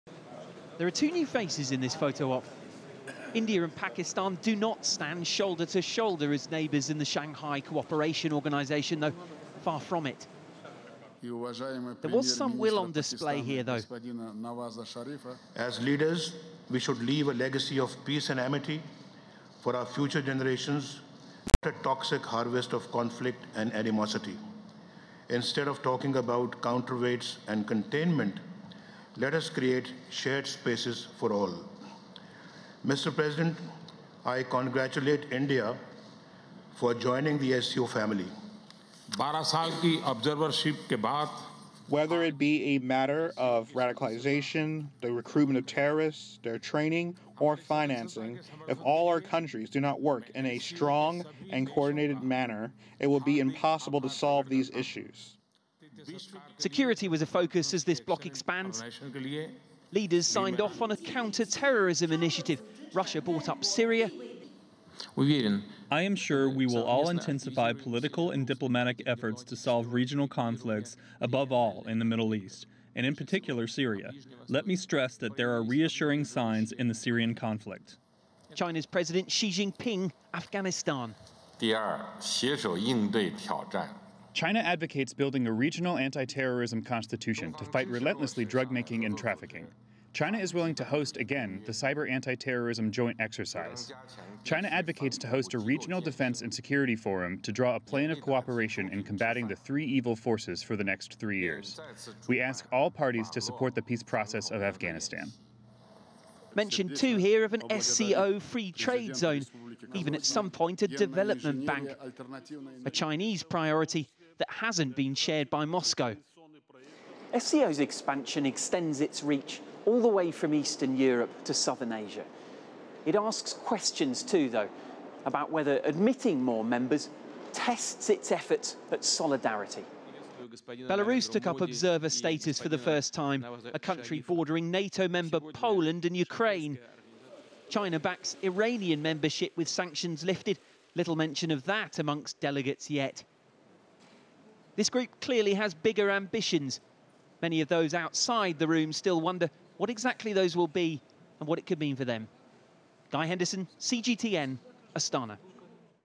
India and Pakistan join SCO, China focuses on improving security
India and Pakistan become members of the Shanghai Cooperation Organisation. This while a summit wraps up focused on security and trade.